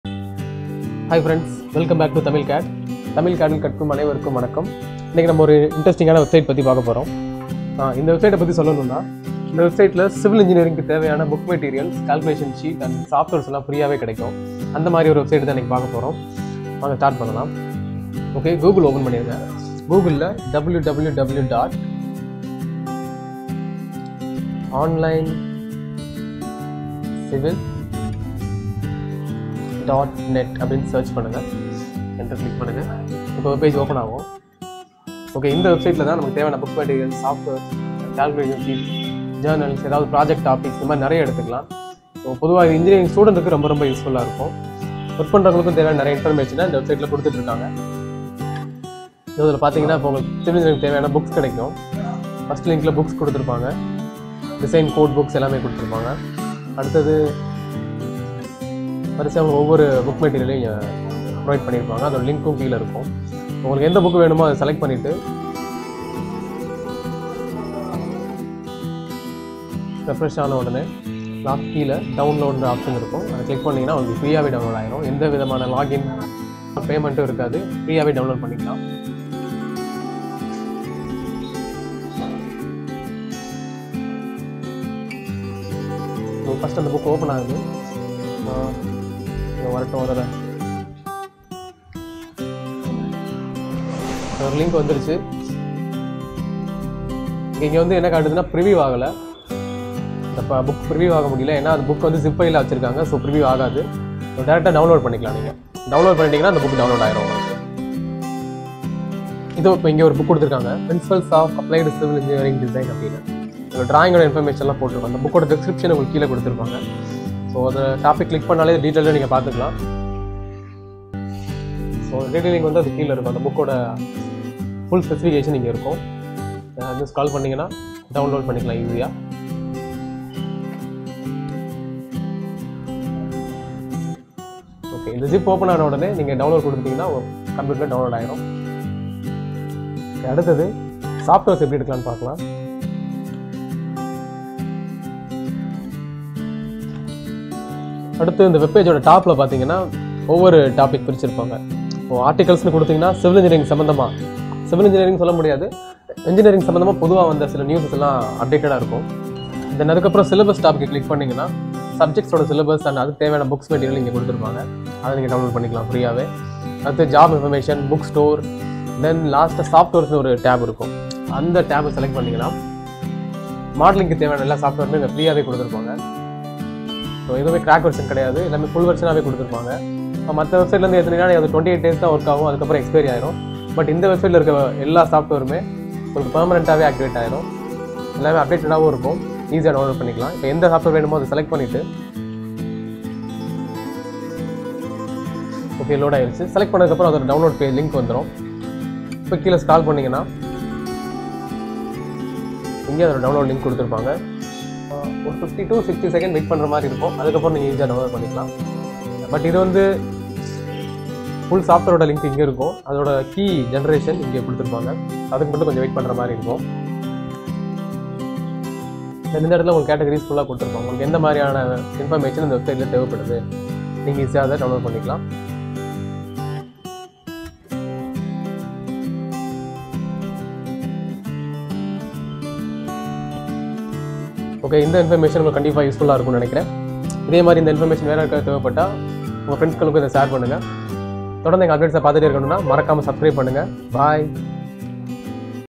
BEST WEBSITE FOR DOWNLOAD SOFTWARE, SPREAD SHEET (EXCEL),BOOKS - தமிழ் CADD Info #4
THIS VIDEO FOR CIVIL ENGINEERING STUDENTS AND WORKING PROFESSIONALS TO PROVIDE A GUIDANCE OF HOW TO DOWNLOAD STANDARD BOOK MATERIALS, JOURNALS, E-BOOKS, MODELING SOFTWARES(AUTOCAD, REVIT, 3DS MAX, ETC.,) IS FULLY FOR FREE...!!!

IF YOU LIKE THIS SHARE THIS WITH YOUR FRIENDS.
 KEEP WATCH FOR BEST CAD TUTORIALS.. ASLO...!!
FOLLOW US TO